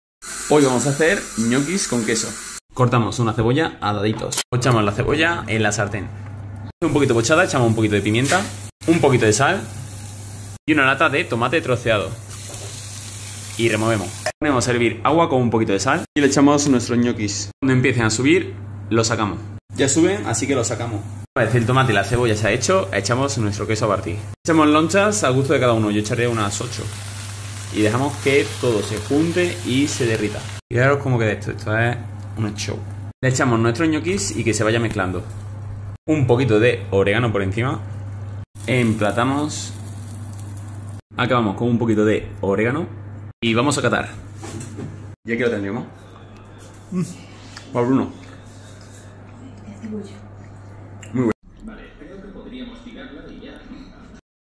Gnoquis o Ñoquis 4 Ingredientes // DCatanFit
Solo necesitas 4 ingredientes.
CEBOLLA, QUESO, GNOQUIS Y TOMATE ¿Lo tienes todo? Pues a empezar con este platazo.